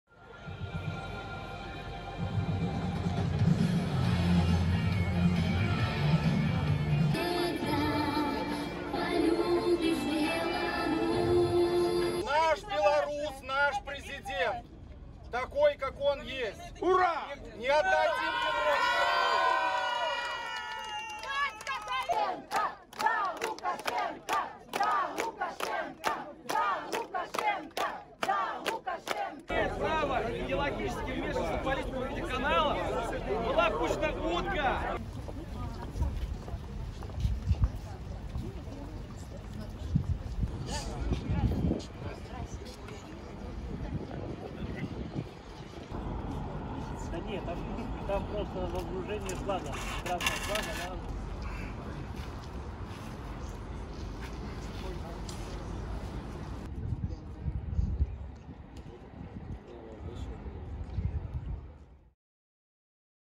Hundreds of pro-government demonstrators gather in Minsk | AFP
Several hundred pro-government supporters are gathered on Independence Square in Belarus' capital Minsk. COMPLETES VIDI1WJ5OL_EN